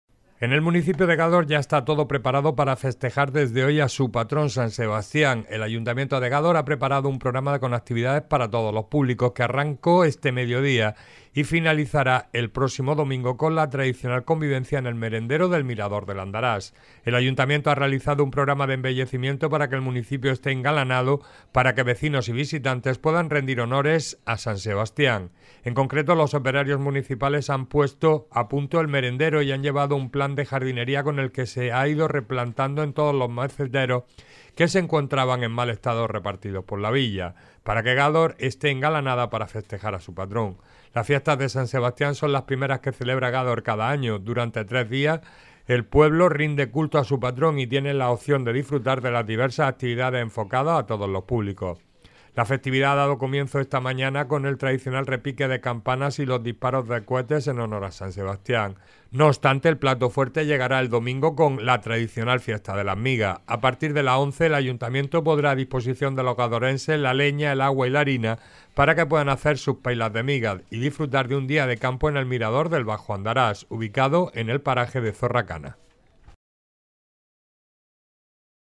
Almería Noticias Canal 28 Tv - Gádor, fin de semana de ocio en honor a San Sebastián
En el municipio de Gádor ya está todo preparado para festejar desde hoy a su Patrón, San Sebastián. El Ayuntamiento de Gádor ha preparado un programa con actividades para todos los públicos que arrancará este mediodía y finalizará el próximo domingo con la tradicional convivencia en el 'Merendero del Mirador del Andarax'. El Ayuntamiento ha realizado un programa de embellecimiento para que el municipio esté engalanado para que vecinos y visitante puedan rendir honores a San Sebastián.